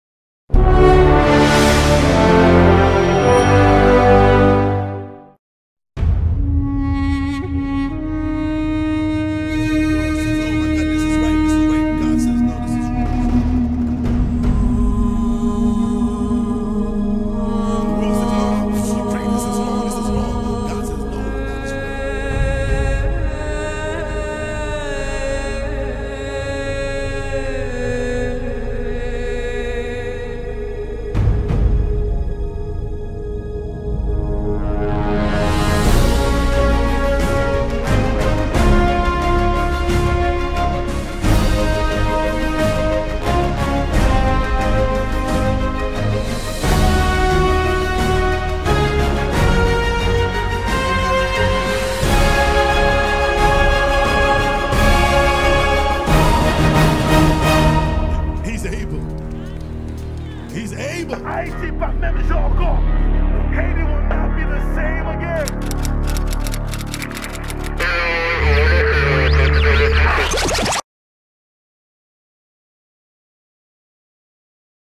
Dickson Guillaume Ministries Presents... Bootcamp 11'... Above The Ashes
As we pass the one year mark of the earthquake in Haiti, where a quarter of a million lost there lives on or as result of... We Will Continue to trust and believe that God will heal Haiti and Restore her back the the pearl she once use to be.

Dickson Guillaume and Haitian Mass once again presents... Bootcamp 11'  with this years theme "Above The Ashes". A week of anointed music and powerful word. Youth leaders, Youth pastors and fellowships ALL OVER... Get Ready for the ultimate spring break Youth Conference... BOOTCAMP 11'... ABOVE THE ASHES.